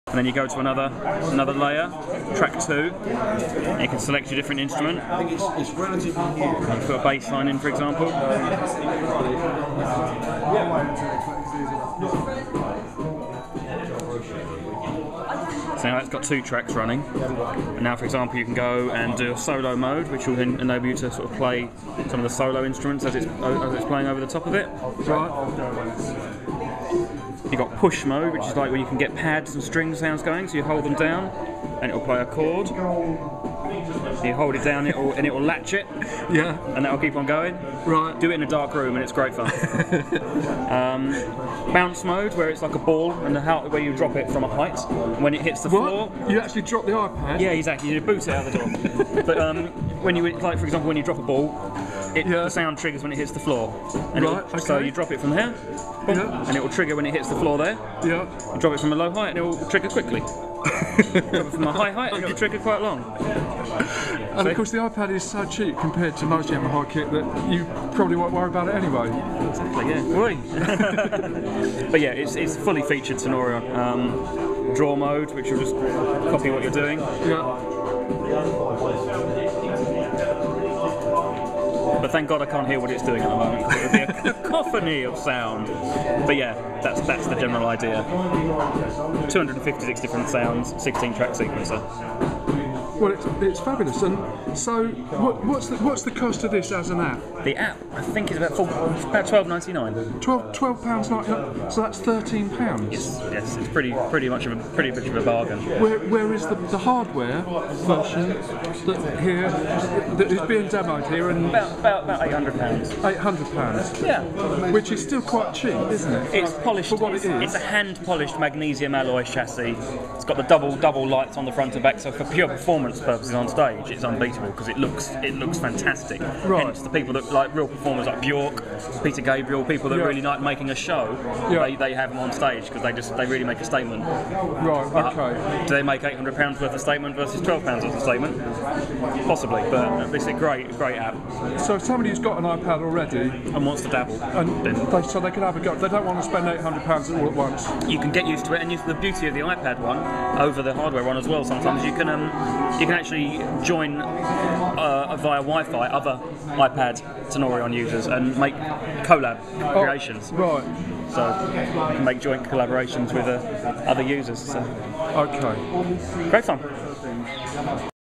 Tenori-on as iPad App
Yamaha showed their full range of equipment at the A2D Music Expo in Exeter Pheonix. The iPad App costs less than twenty GB pounds so has a wider appeal than the dedicated hardware costing arounde 800 GB pounds. See also by.polar.bares at Phoenix Bar Exeter for performance with the proper equipment. There were those who stated the extra cost is well worth it but it will also be interesting to see how far apps can go.